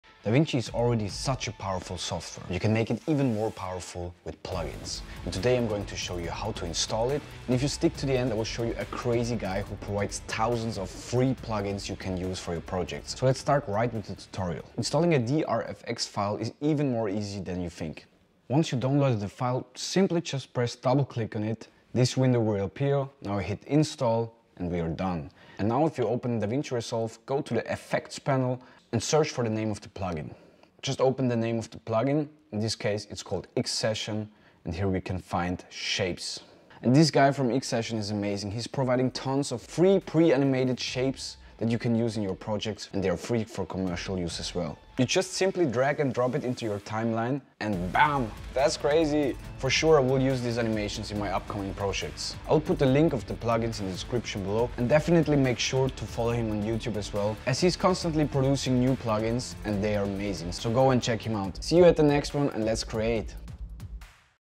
How to Easily Install DaVinci Resolve DRFX Plugins
Learn how to easily install DaVinci Resolve DRFX plugins in this tutorial. DRFX plugins from @XSession  can enhance your editing experience and bring your projects to the next level. Follow along as I guide you through the installation process, enabling you to take full advantage of these powerful tools.